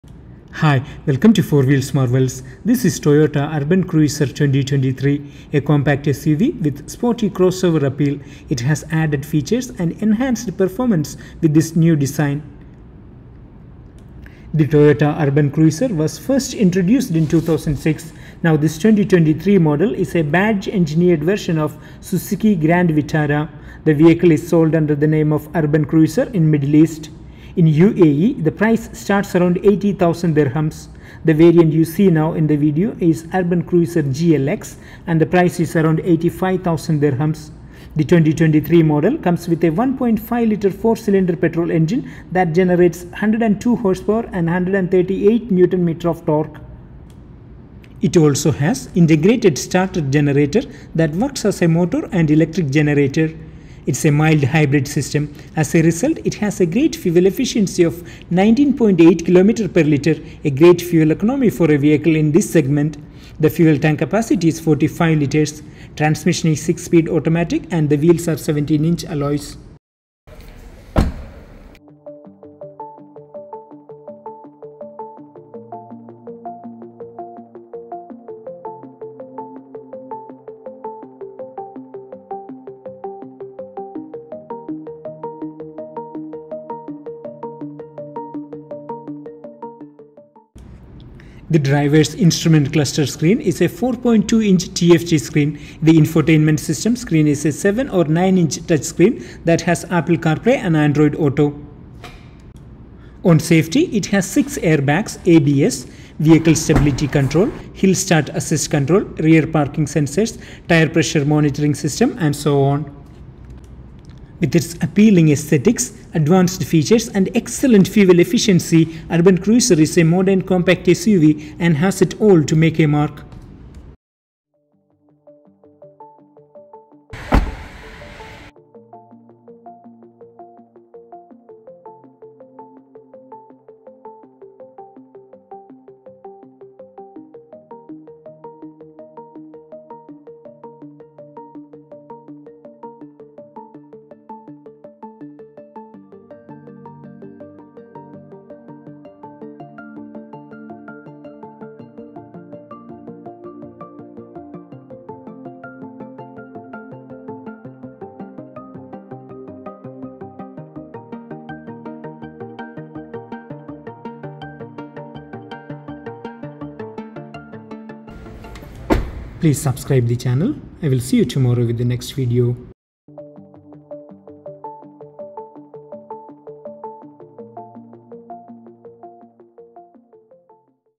Toyota Urban Cruiser is Suzuki too !
Toyota Urban Cruiser 2023: A badge engineered version of excellent Suzuki grand Vitara.